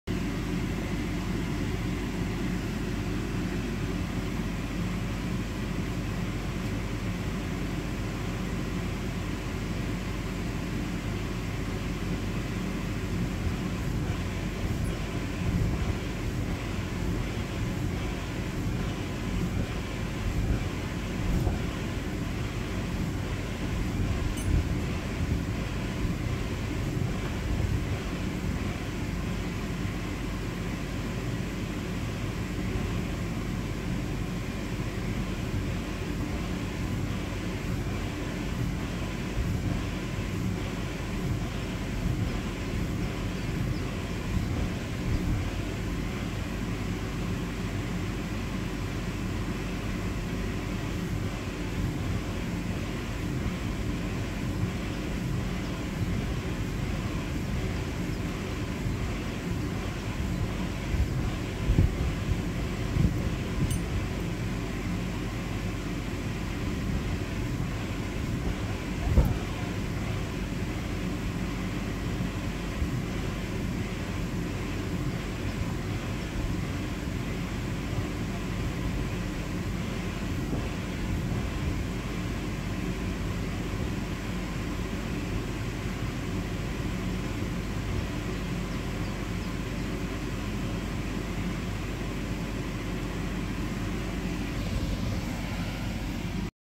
cow bounce house blower view
Watch the back of the bounce house when i jump at pancakehouse "midden op t landt" . it is an larger model and not so old so i had to do my best.